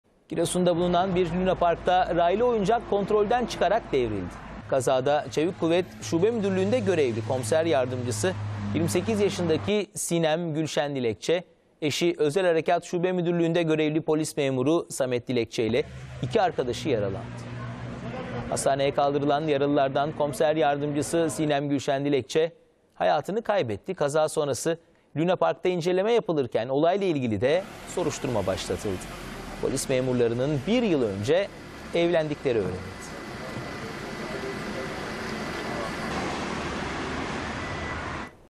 Giresun'da lunapark kazası: 1 ölü, 3 yaralı | A Haber
Giresun'da lunaparkta meydana gelen kazada bir polis hayatını kaybetti, biri polis 3 kişi ise yaralandı.

Alınan bilgiye göre, Hacı Hüseyin mahallesinde bulunan bir lunaparkta raylı oyuncak henüz belirlenemeyen bir nedenden dolayı kontrolden çıkarak devrildi. Meydana gelen kazada Çevik Kuvvet Şube Müdürlüğü'nde görevli Komiser Yardımcısı 28 yaşındaki Sinem Gülşen Dilekçe, eşi Özel Harekat Şube Müdürlüğü'nde görevli Polis Memuru Samet Dilekçe (31) ile beraber Şevval Akbaş (17) ve Fatma Akbaş (39) yaralandı. Giresun Eğitim ve Araştırma hastanesine kaldırılan yaralılardan tüm müdahalelere rağmen Şube Müdürlüğü'nde görevli Komiser Yardımcısı 28 yaşındaki Sinem Gülşen Dilekçe hayatını kaybetti.

Giresun Valisi Enver Ünlü hastanede yaralıları ziyaret ederken, emniyet mensuplarına da başsağlığı diledi. Olayla ilgili açıklamalarda bulunan Vali Enver Ünlü, “Üzücü bir kaza sonucu 4 vatandaşımızın ikisi emniyet mensubu olmak üzere yaralandılar. Doktor arkadaşlarımızın büyün müdahalelerine rağmen komiser yardımcısı evladımızı maalesef kaybettik.

Hepimizin başı sağ olsun. İdari tahkikat, adli tahkikat devam ediyor. Maalesef personelimizi kaybettik kaza sonrası. Başsağlığı diliyorum, başta emniyet camiası olmak üzere, sevenlerine, emniyet teşkilatına, kıymetli ailesine başsağlığı diliyorum. Başsağlığı dileklerimi iletmek için geldim. Arkadaşlarımız ellerinden geleni yaptılar ama maalesef genç yaşta bir personelimizi kaybettik. Acımız büyük üzgünüz. Diğer konularla ilgili şu anda bir şey söylemem doğru olmaz. Yaralılarımızın durumu iyi. Rahmetli komiser yardımcımızın eşi de biraz önce taburcu oldu. Ama maalesef bir personelimizi kaybettik” açıklamasında bulundu.